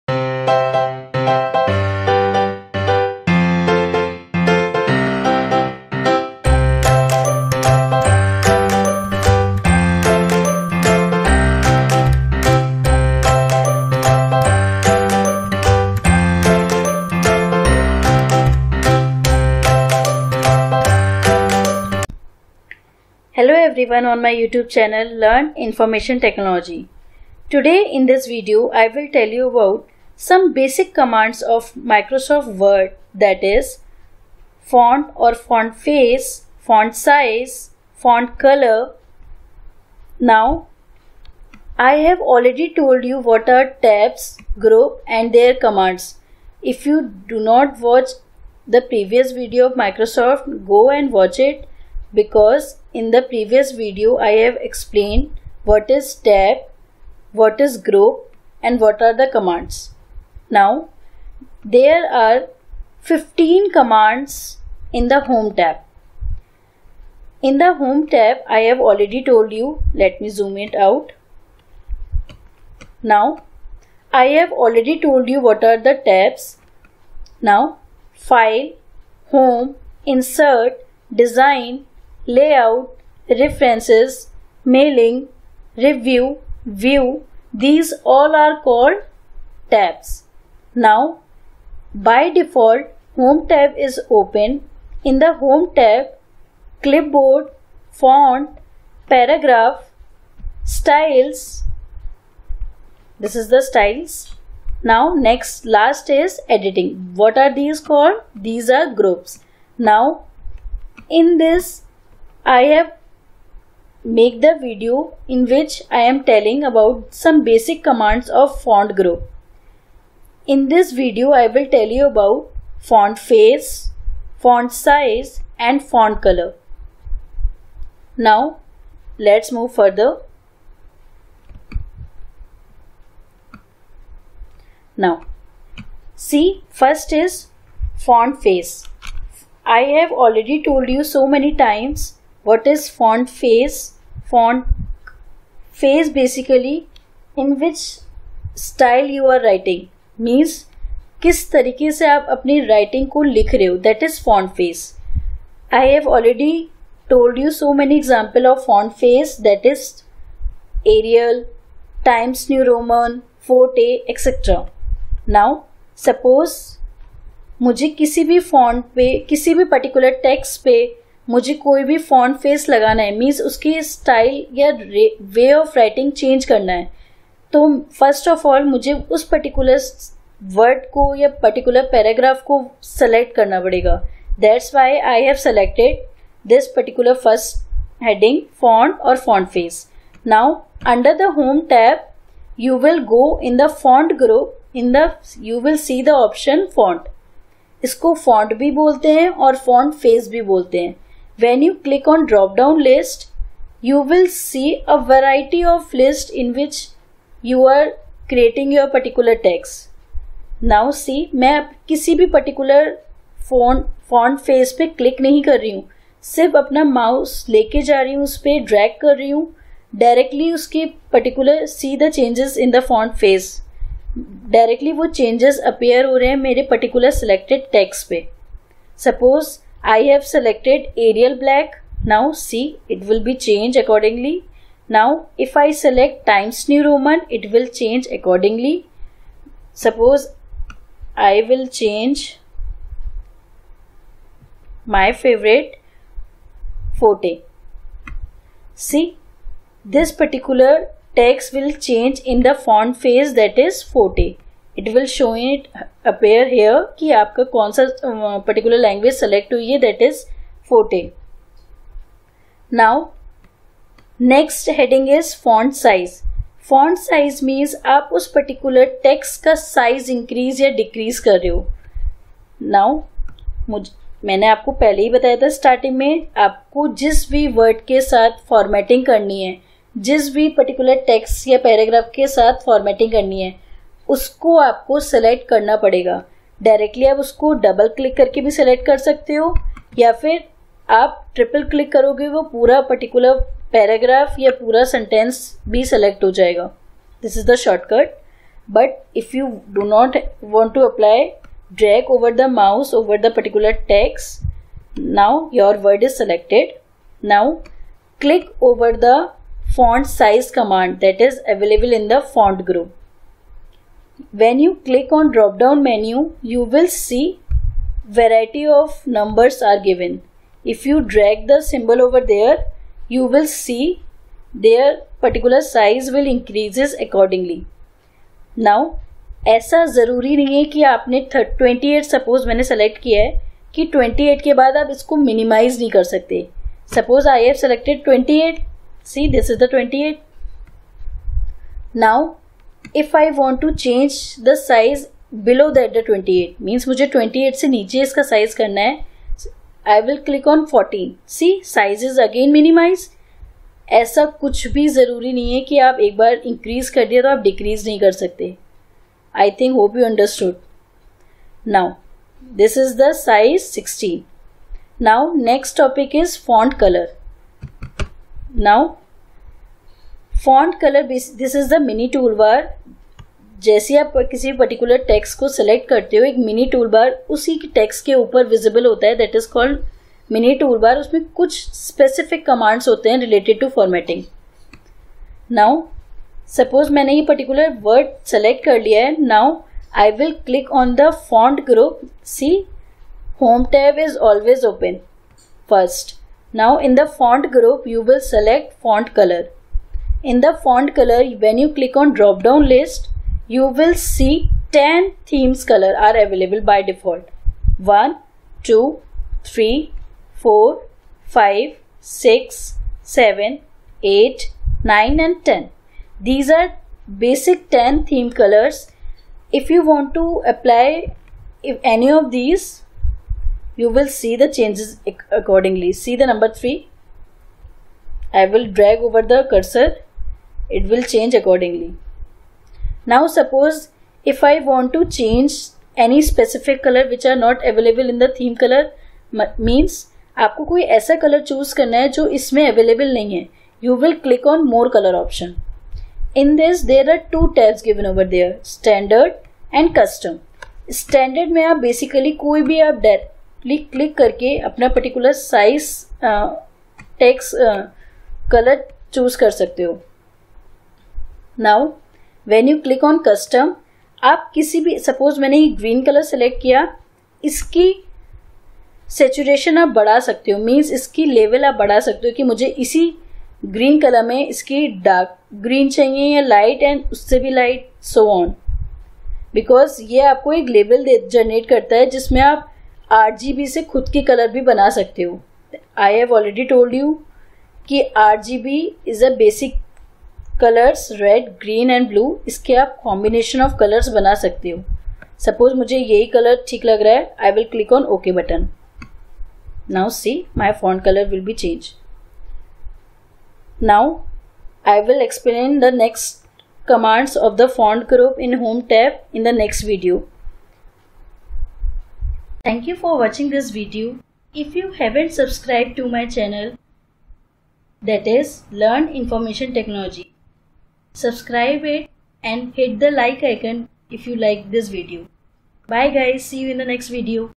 Font Group in MS Word- Part 1 || Formatting of text
In this video you will learn about some basic commands of Font Group in Home Tab. In this video i will also give live demo to apply for formatting of text.

✓What is font or Font face?
✓How we can apply font face to any text or paragraph?
✓What is Font size?
✓How we can apply font size to any text or paragraph?
✓What is Font color?
✓How we can apply any font Color to text or paragraph?

In this video I have use some terms like tabs, ribbon, group, if you have any confusion
✓What is tabs?
✓What is Group?
✓What is ribbon?
i will give the link of that even in description box or
even in (i) button present at top right corner.
 If you haven't watch that video go any watch it.

comment me in the comment section for requesting any new video making request on the topic.